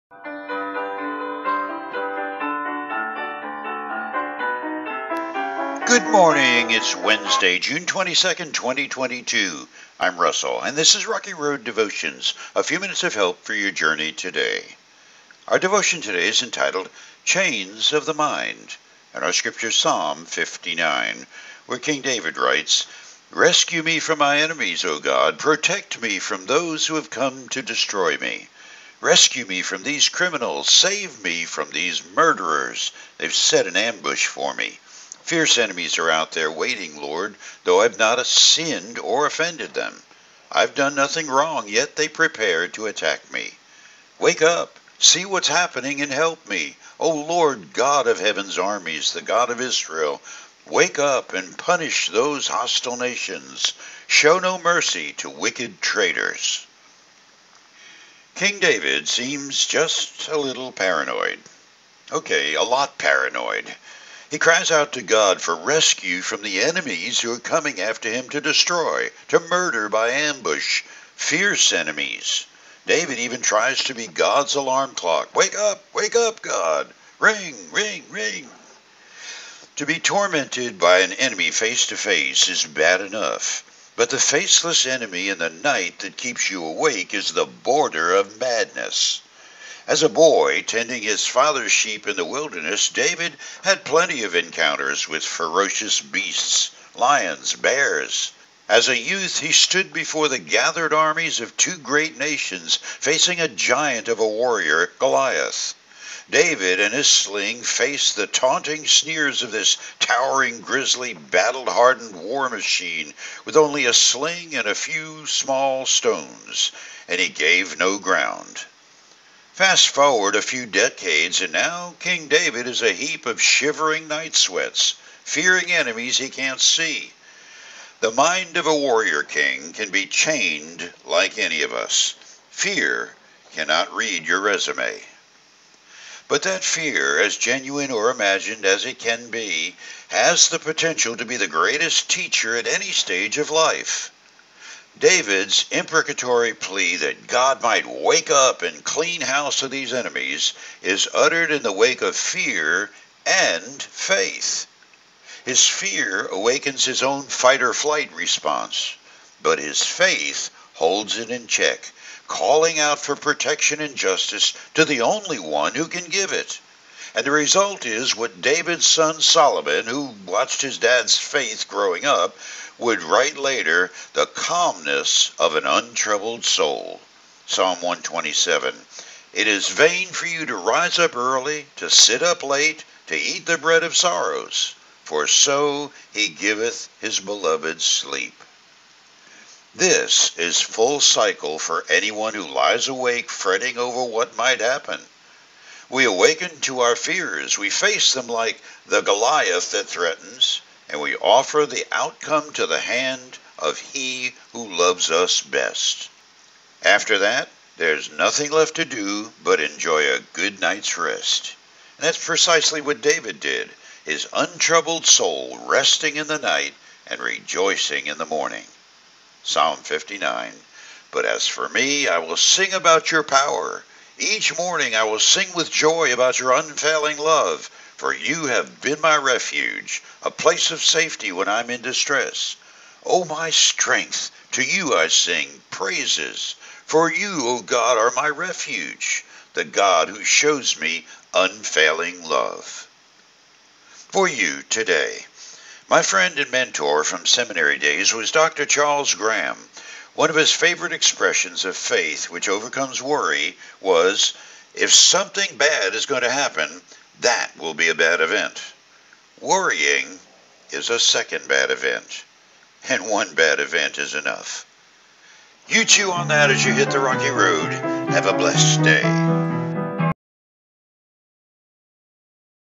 062222 Chains of the MIND
King David seems just a little paranoid (OK…a LOT paranoid).  He cries out to God for rescue from the “enemies” who are coming after him to destroy, to murder by ambush…fierce enemies!  David even tries to be God’s alarm clock…wake up, wake up, God…RING….RING….RING!